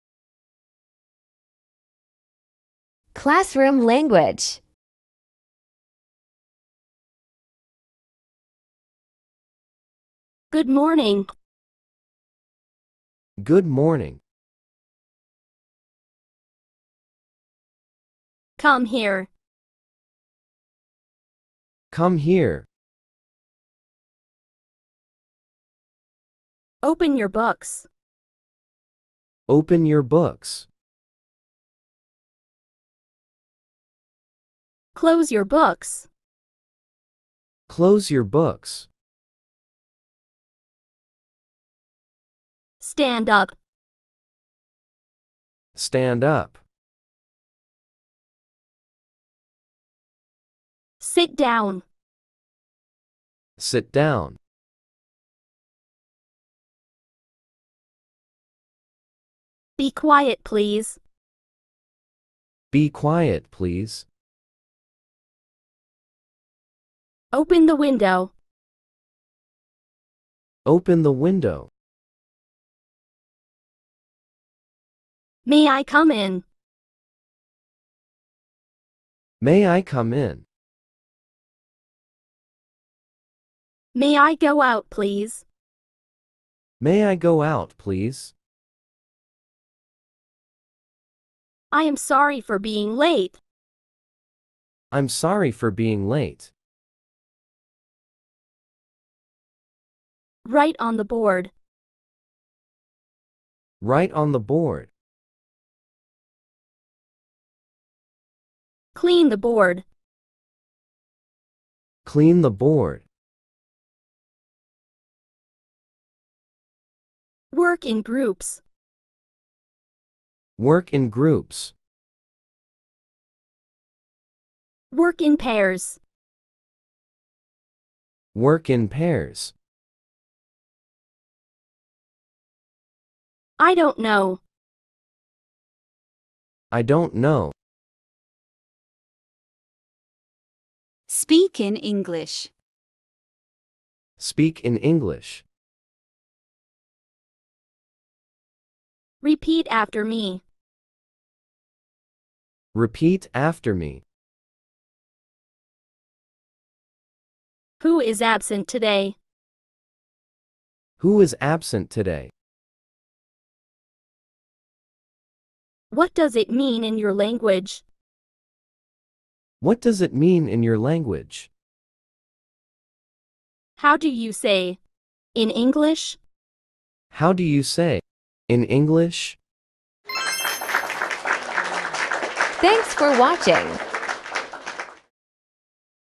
CLASSROOM LANGUAGE
A presentation for English language learners and teachers. Moreover, it can be used in the classrooms to teach classroom language.